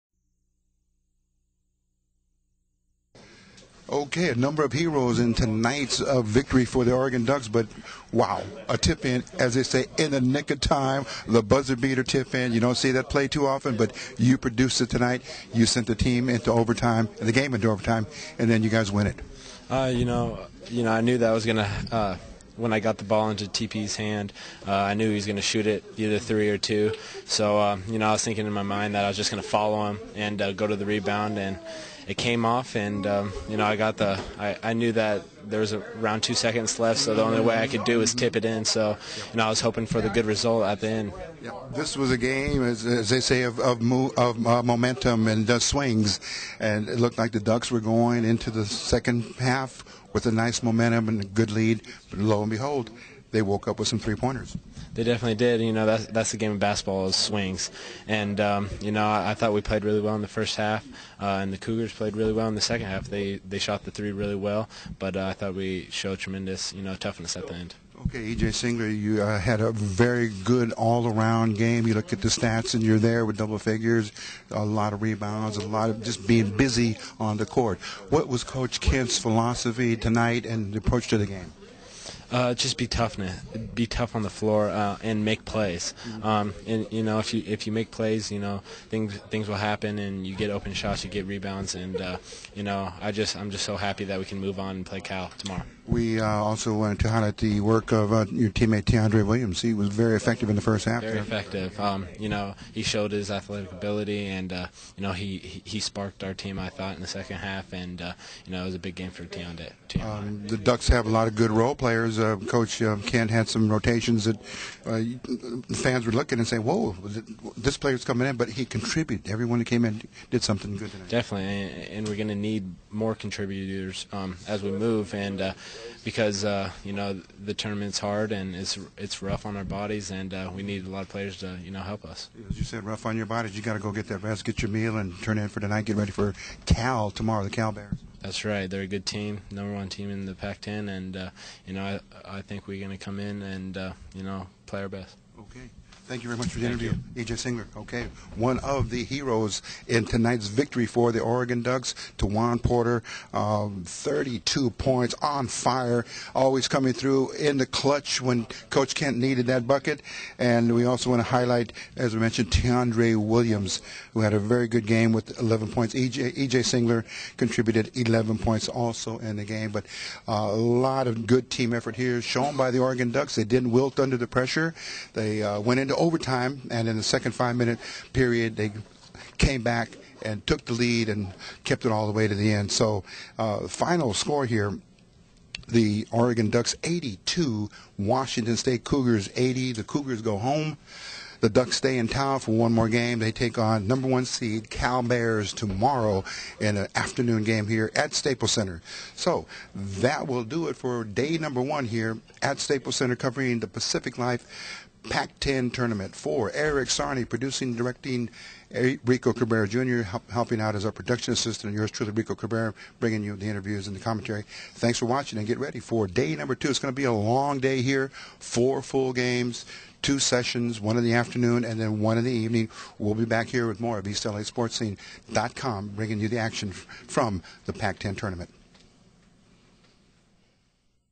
E.J Singler - Post Game Interview vs Washington st - Pac -10 Tournament 2010
Pac-10 Tournament Round #1..Final in Overtime ..Oregon Ducks 82. Washington state 80..